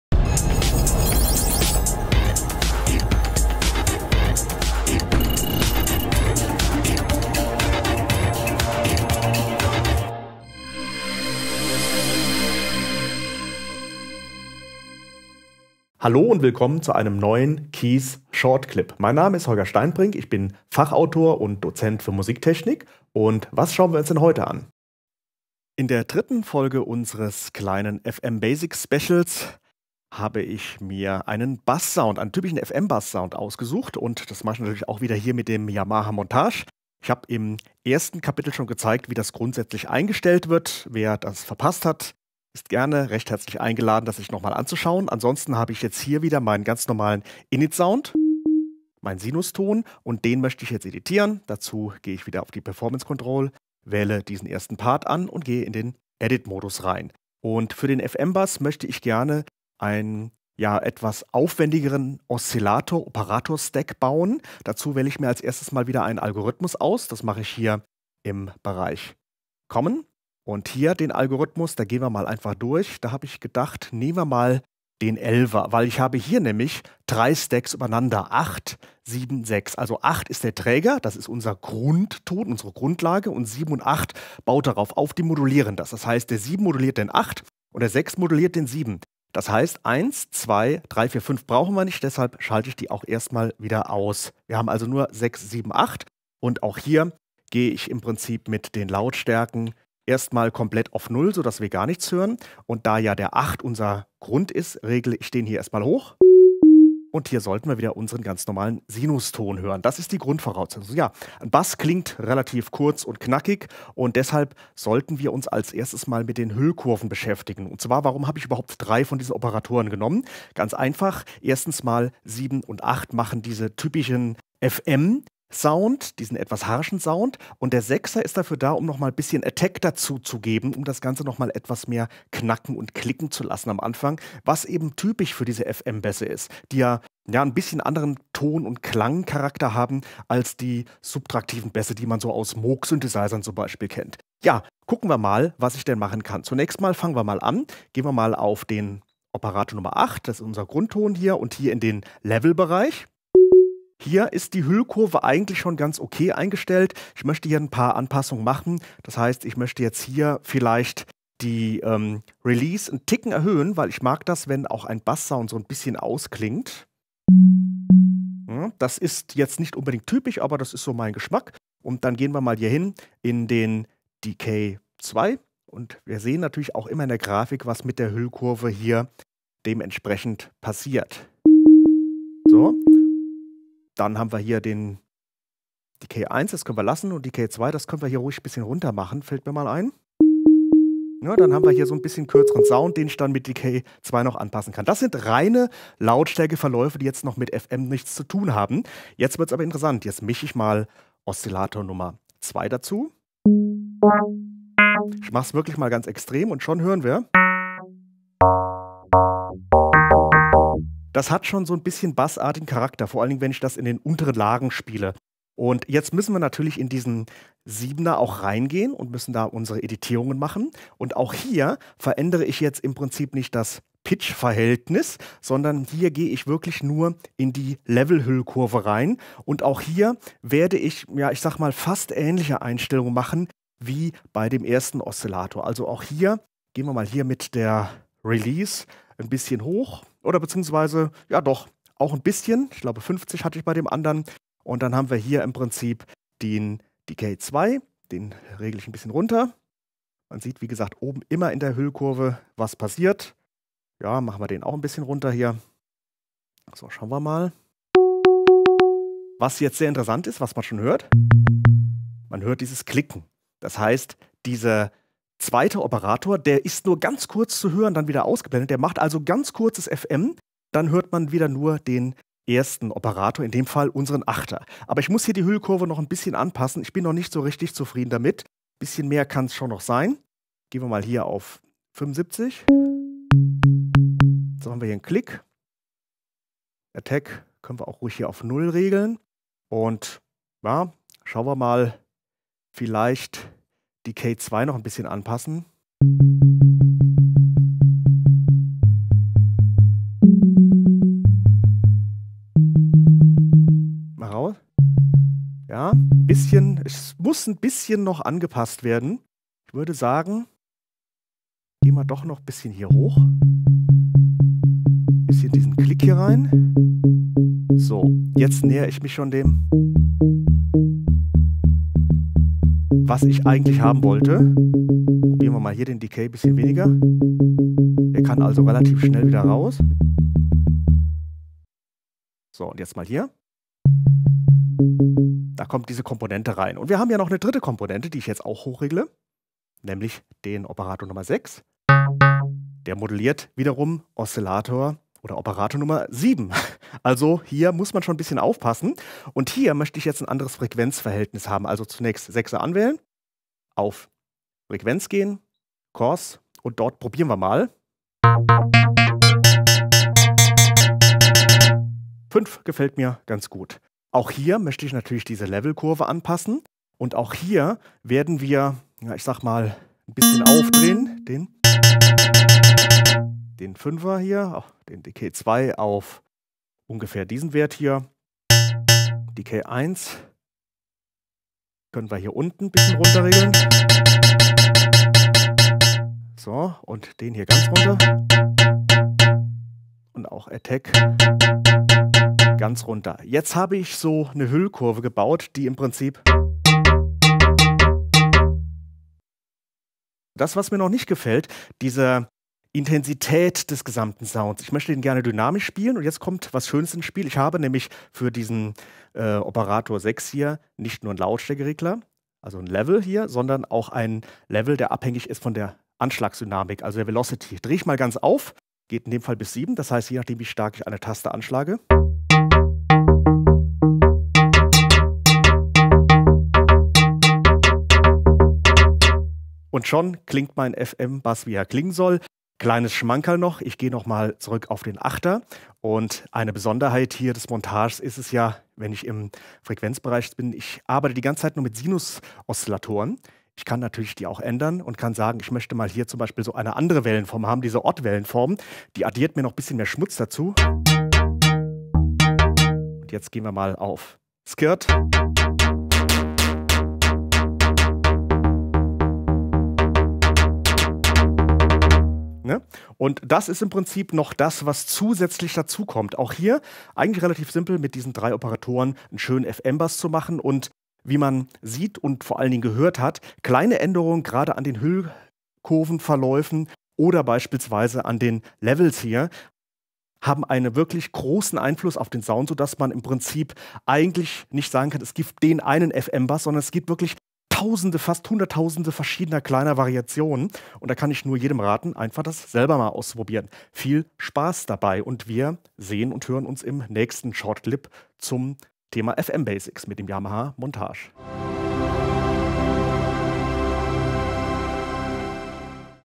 FM-Synthese einfach erklärt, Teil 3 (FM-Bass) – KEYS Short Clips
Im dritten Clip unserer Yamaha-Montage-Reihe erstellt KEYS-Autor Holger Steinbrink einen FM-Bass-Sound.

Weitere Infos zum Yamaha Montage:
goo.gl/w2u4xg